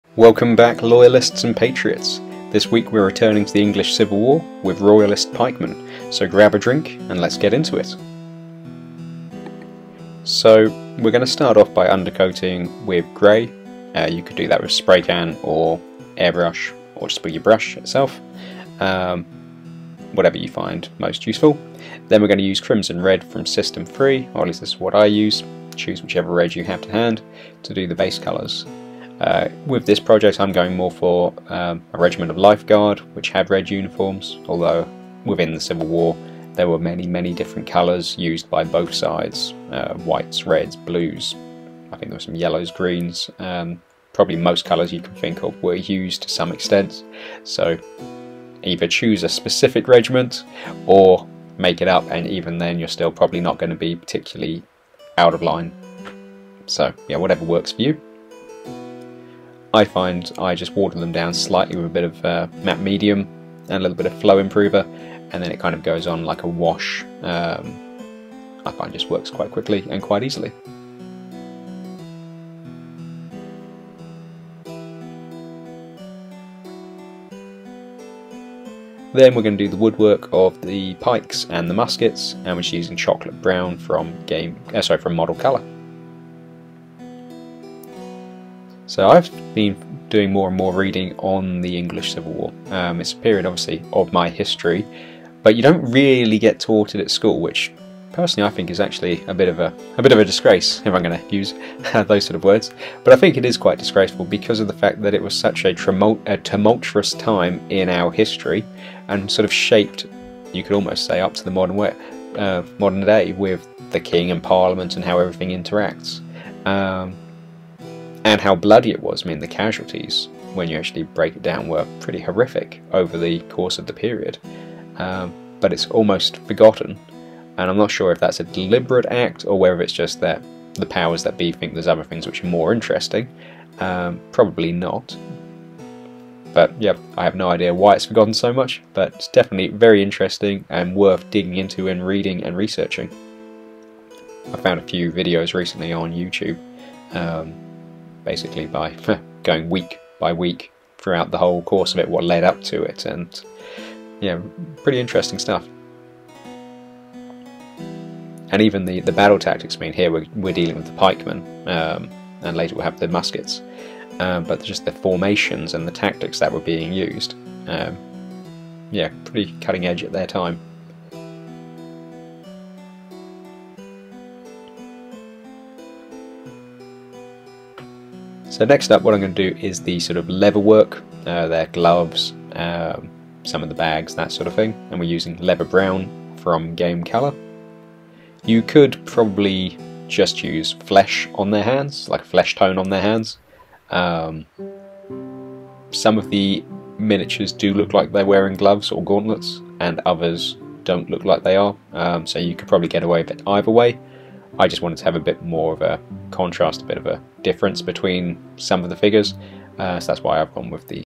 Epic Battles Pike and shotte Royalist pikemen Easy Speed painting tutorial Warlord games
In this video, we present a captivating tutorial on painting English Civil War Royalist Pikemen. Immerse yourself in the brushstrokes of loyalty as we guide you through the process of bringing these historical miniatures to life.

Our expert painters carefully demonstrate the techniques required to paint these Royalist Pikemen accurately. From selecting the ideal color palette to applying intricate details, you'll learn step-by-step how to achieve stunning results. Whether you're a seasoned painter or just starting out, this tutorial offers valuable tips and insights for everyone.

In this video, we prioritize authenticity, ensuring that every tiny detail reflects the true essence of the period. We delve into historical resources to grasp the essence of the Royalist Pikemen's attire, weaponry, and distinctive features. By following our guidance, you'll be able to create an impressive army that accurately represents the English Civil War era.

Join us on this artistic journey, as we celebrate the Royalist Pikemen's loyalty with every brushstroke. With our easy-to-follow instructions and creative inspiration, you'll soon master the art of painting these remarkable historical figures.

Don't miss this opportunity to improve your painting skills while immersing yourself in the captivating world of the English Civil War.